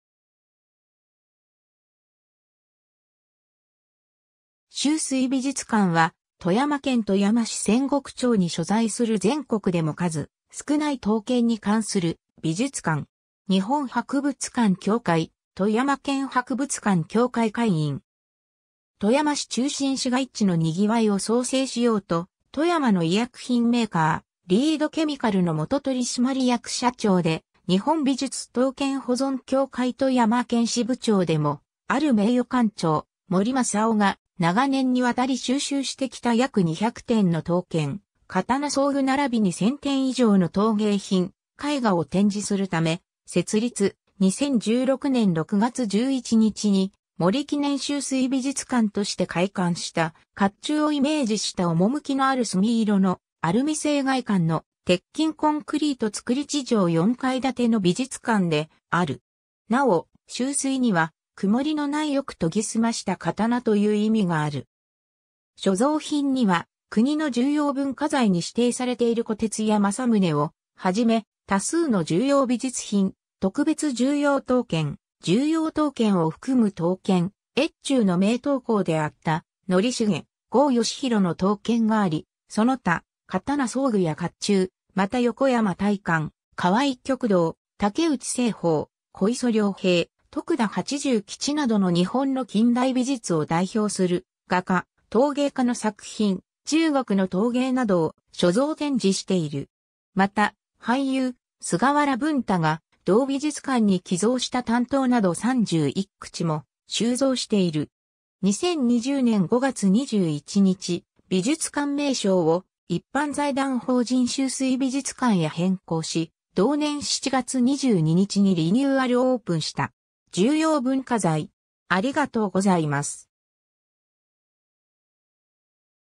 秋水美術館
秋水美術館（しゅうすいびじゅつかん、SHUSUI MUSEUM OF ART）は、富山県富山市千石町に所在する全国でも数少ない刀剣に関する美術館。
日本博物館協会、富山県博物館協会会員。
 富山市中心市街地の賑わいを創生しようと、富山の医薬品メーカー「リードケミカル」の元取締役社長で日本美術刀剣保存協会富山県支部長でもある名誉館長 森政雄が、長年に渡り収集してきた約200点の刀剣、刀装具並びに1,000点以上の陶芸品・絵画を展示するため設立、2016年（平成28年）6月11日に森記念秋水美術館として開館した、甲冑（鎧）をイメージした趣のある墨色のアルミ製外観の、鉄筋コンクリート造り地上4階建ての美術館である。
なお「秋水」には曇りのないよく研ぎ澄ました刀という意味がある。
 所蔵品には国の重要文化財に指定されている虎徹や正宗をはじめ、多数の重要美術品、特別重要刀剣、重要刀剣を含む刀剣、越中（富山）の名刀工であった則重、郷義弘の刀剣があり、その他、刀装具や甲冑（鎧兜）、また横山大観、川合玉堂、竹内栖鳳、小磯良平、徳田八十吉などの日本の近代美術を代表する画家・陶芸家の作品、中国の陶芸などを所蔵展示している。
 また、俳優菅原文太が同美術館に寄贈した短刀など31口も収蔵している。
 2020年（令和2年）5月21日、美術館名称を一般財団法人 秋水美術館へ変更し、同年7月22日にリニューアルオープンした。
 重要文化財